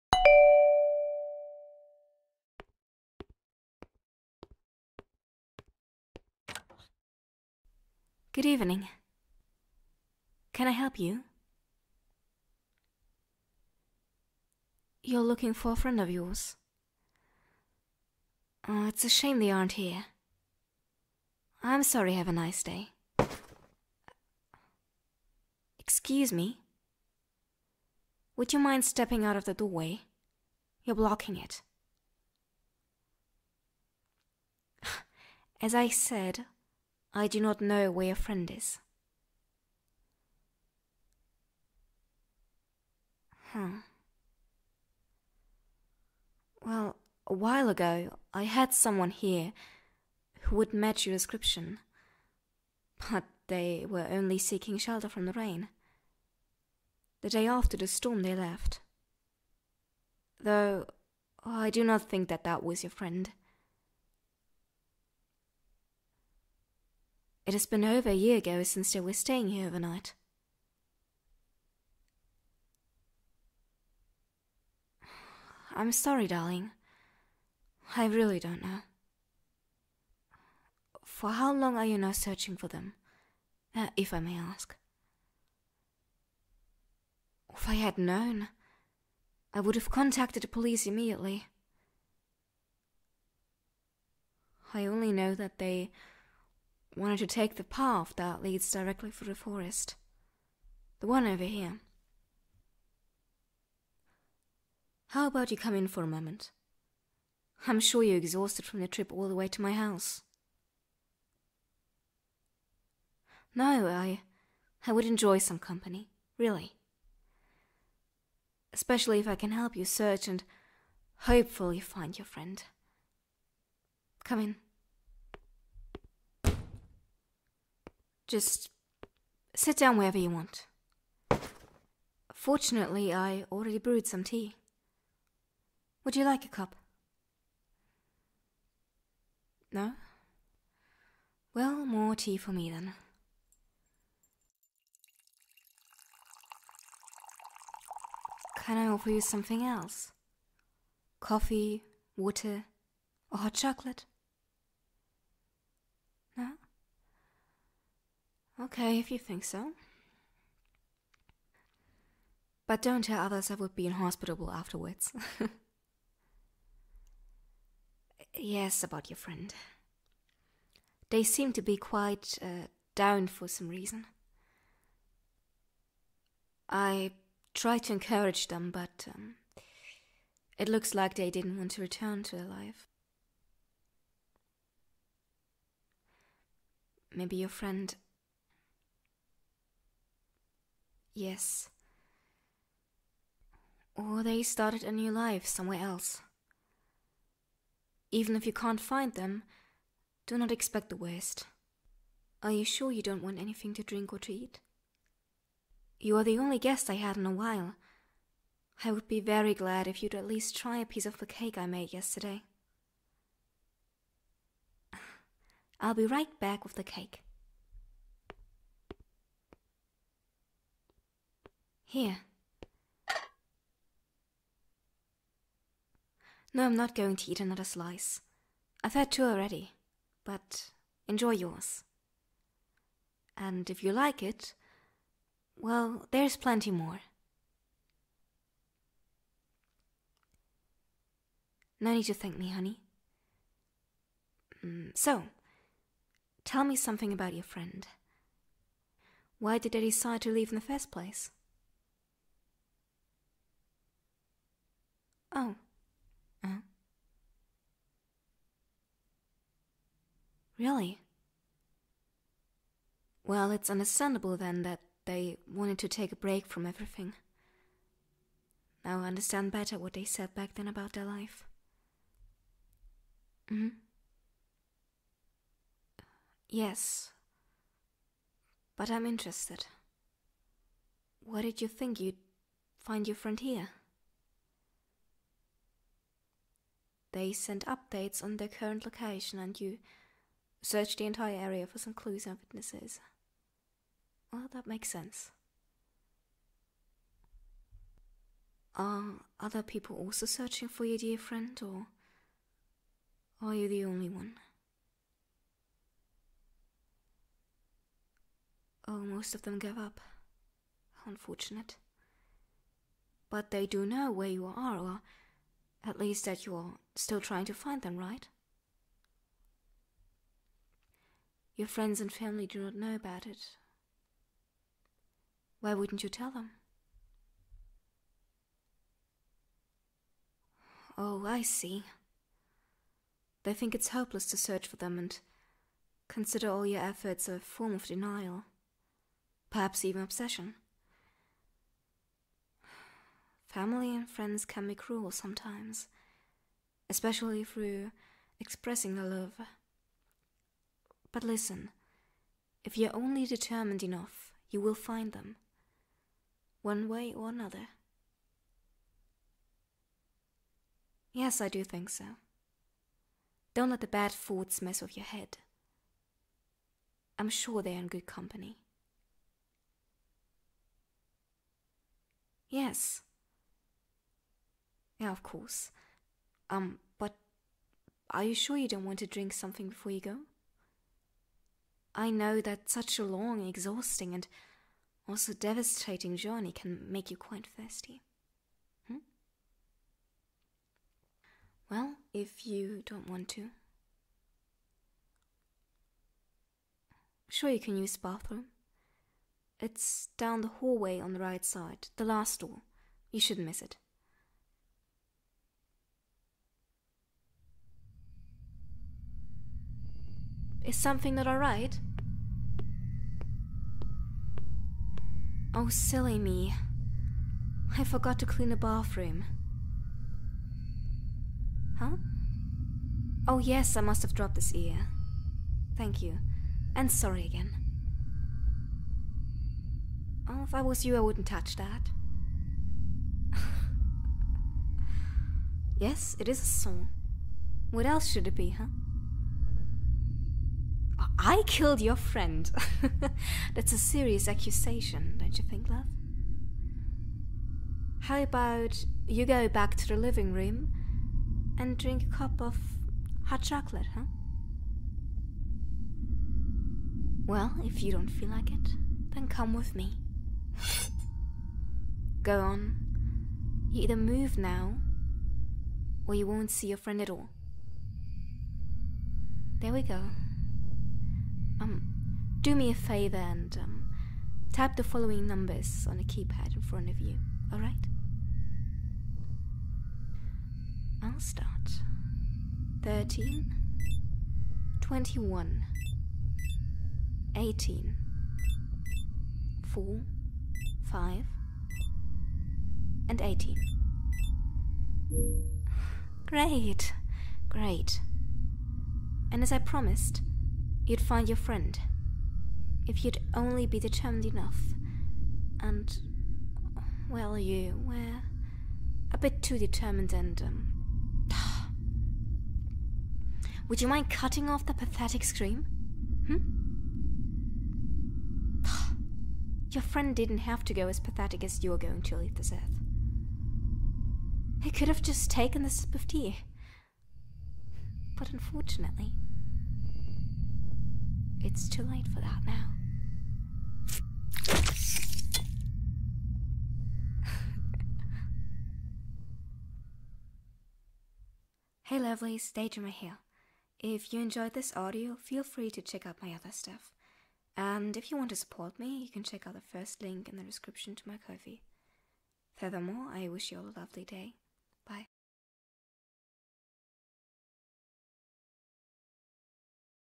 Strange but kind Woman helps You find Your missing Friend (F4A)/(F4M)/(F4F)
After one year of searching your missing friend, you find yourself in front of the door of a kind woman. She is willing to help you find your friend, who sought shelter in her house around the time they went missing. Though she is incredibly friendly after warming up to you, you can't ignore the weird atmosphere.

Thanks for listening! Because of the channel's anniversary, I thought I'd make a sequel to the first video on this channel. It would help this channel a lot. Furthermore, I wish you a lovely day!

Audio, script and editing by me.
Picture belongs to the artist.